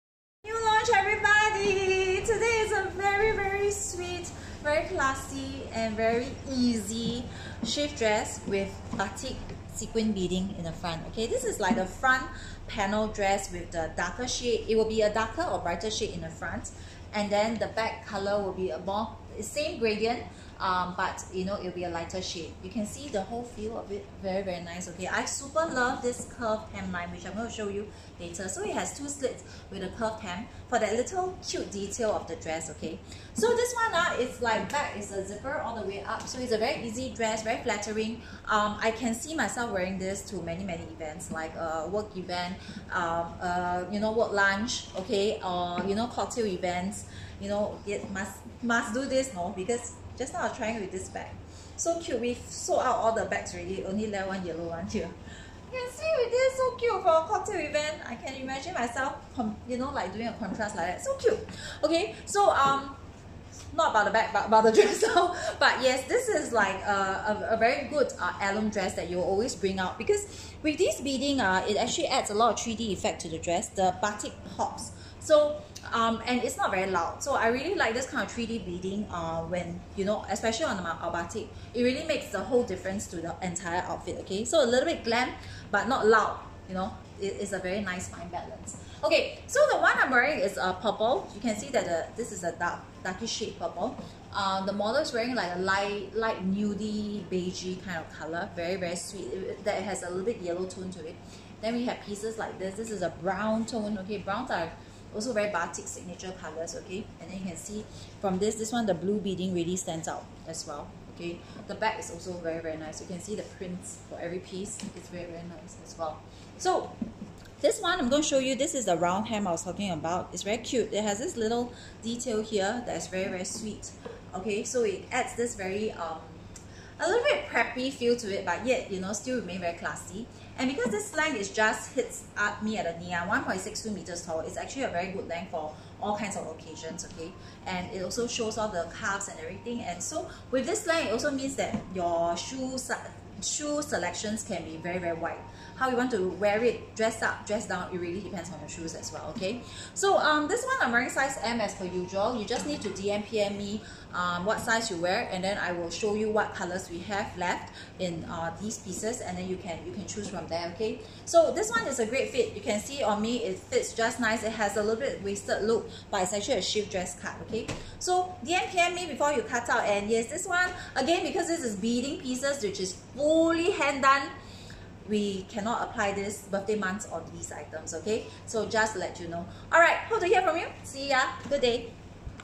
Aria Beaded Batik Dress
Beaded panel shift dress in gradient tones and curved hemline for a feminine, classy statement.